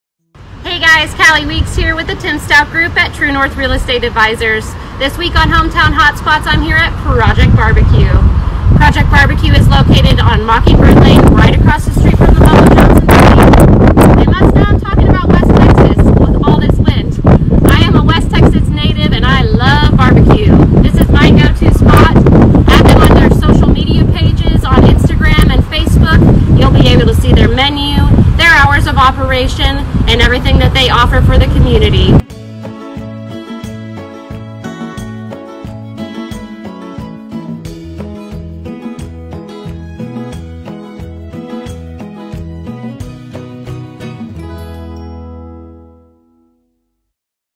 Hometown Hotspot Project BBQ!
This week on Hometown Hotspots is Project BBQ
This place is amazing! If you enjoy bbq, this is the place to go. 🤤